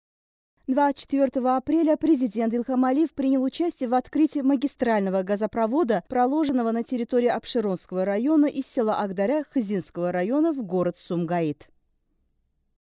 Президент Ильхам Алиев принял участие в открытии магистрального газопровода
Президент Ильхам Алиев принял участие в открытии магистрального газопровода, проложенного в город Сумгайыт